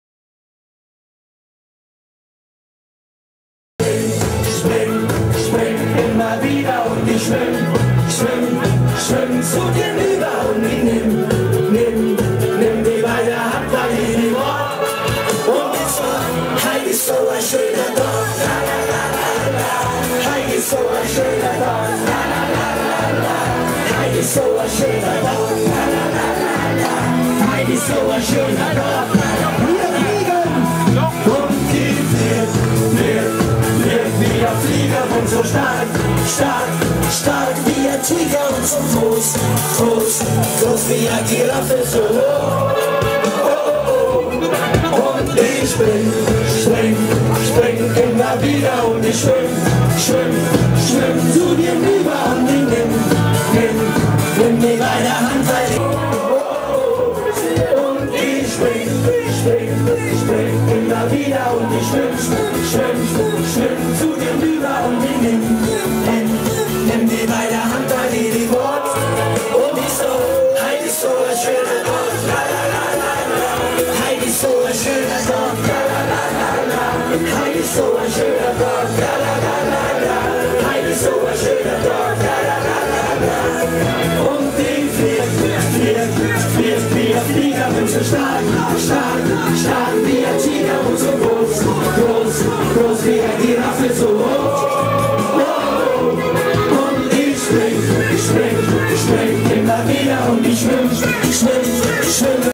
אישגל - סקי דיל - Trofana Alm Apre Ski - SkiDeal
בואו להכיר את חיי הלילה הכי טובים בסביבה. באחריות. אישגל, אוסטריה.
למידע נוסף והזמנת חבילות סקי ובטלפון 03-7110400